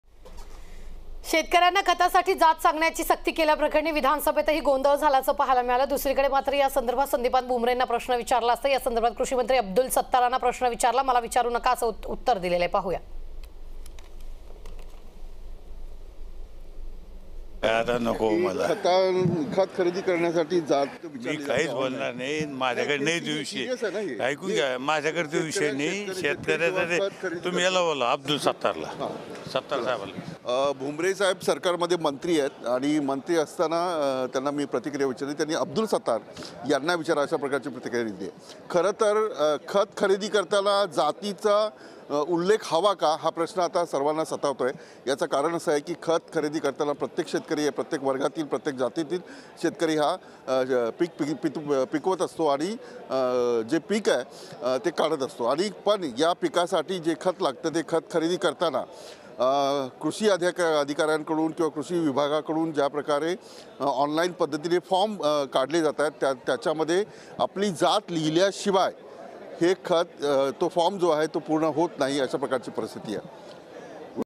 Sandipan Bhumbre On Farmer | सांगलीतील ईपास प्रकरणावर विचारलं आसता भुमरेंची टाळाटाळ
Credit: Political | Utkarsha Nadge /Producer |